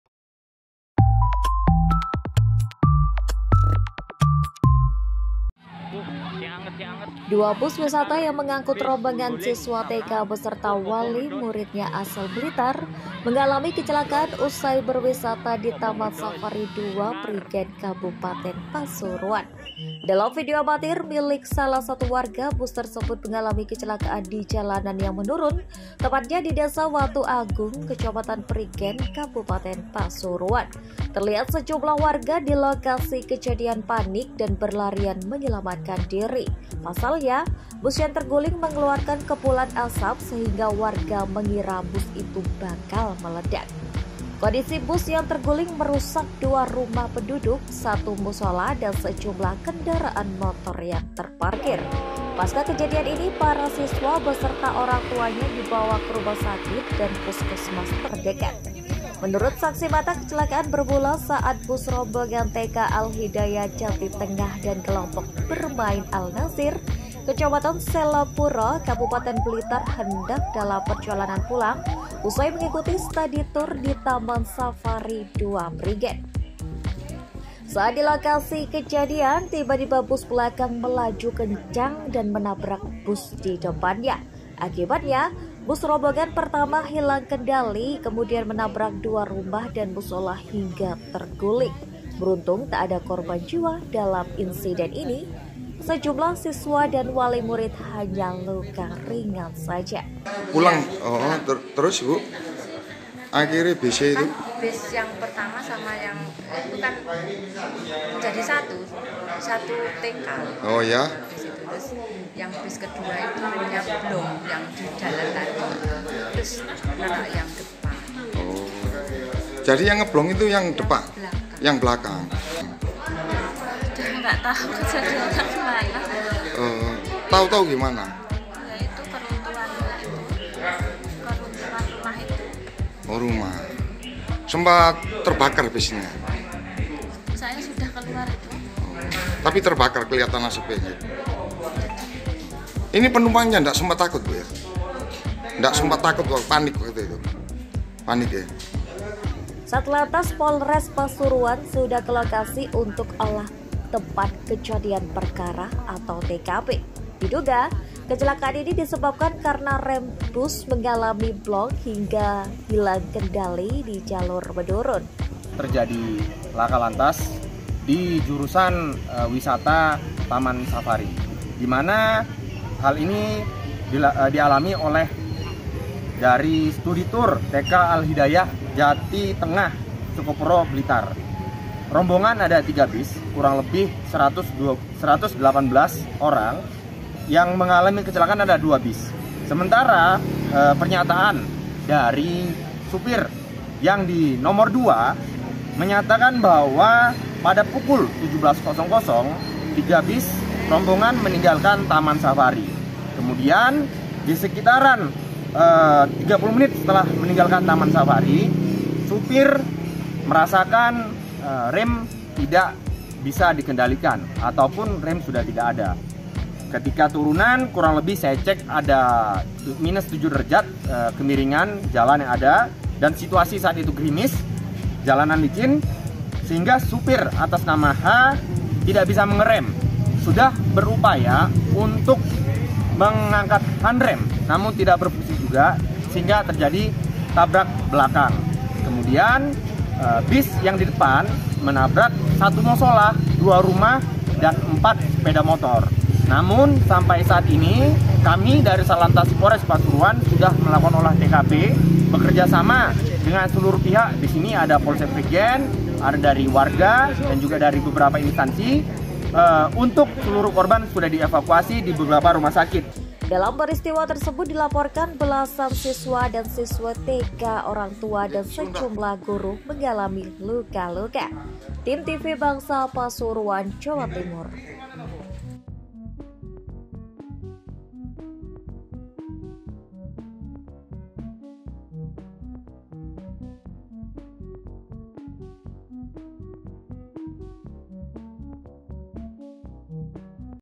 Bus Angkut Rombongan Siswa TK Terguling Tabrak Rumah dan Mushola di Jalur Wisata Prigen Pasuruan
DUA BUS WISATA YANG MENGAKUT ROMBONGAN SISWA TK BESERTA WALI MURIDNYA ASAL BLITAR, MENGALAMI KECELAKAAN USAI BERWISATA DI TAMAN SAFARI 2 PRIGEN, KABUPATEN  PASURUAN.

DALAM VIDEO AMATIR MILIK SALAH SATU WARGA/ BUS TERSEBUT MENGALAMI KECELAKAAN DI JALANAN YANG MENURUN, TEPATNYA DI DESA WATUAGUNG KECAMATAN PRIGEN/ KABUPATEN PASURUAN.

TERLIHAT SEJUMLAH WARGA DI LOKASI KEJADIAN PANIK DAN BERLARIAN MENYELAMATKAN DIRI.

Meski buahnya tergolong kecil, namun durian ini memiliki citarasa yang tidak dimiliki varietas durian lainnya.